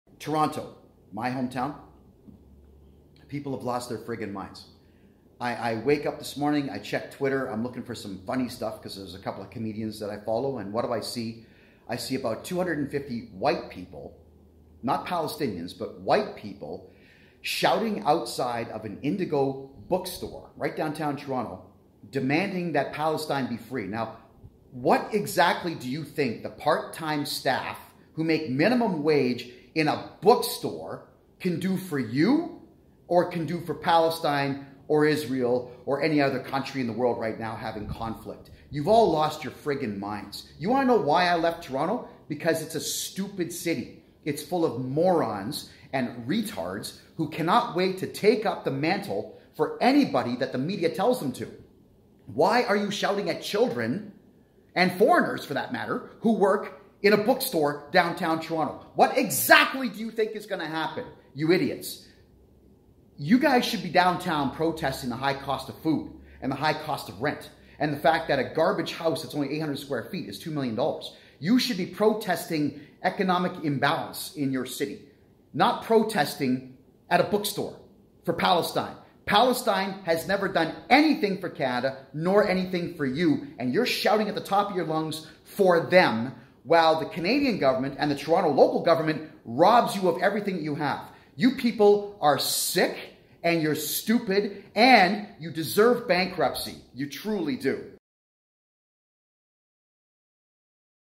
TORONTONIANS ATTACKING !ndigo EMPLOYEES OVER PALESTINE
Morons in Toronto who are not Palestinian and who were not born in the Middle East are standing outside of indigo bookstores screaming at the top of their lungs at the staff in the stores telling them to do something about the Palestine debacle.

What the fuck do people in Toronto expect part-time bookstore staff to do about Israel or Palestine or any other War taking place in the world? People in Toronto have become the stupidest idiots in the world. Pick your battles better. Fighting against a bookstore is never going to end a war in the Middle East and nothing that you say or do in Canada or any other country is going to end this Skirmish taking place between two countries who will fight each other until the end of time.

Maybe you should ask yourself, why do you actually care about this Palestine thing anyway? What is Palestine ever done for you or for Canada or for anyone else for that matter?

You should be protesting what your Canadian government is doing to Canadians but you're just so fucking stupid that you can't help yourselves so by all means Shout at kids working in a bookstore because that's going to make your life just so much better, isn't it?

Watch the Kevin J Johnston show every Tuesday and Thursday at 9:00 p.m. eastern Time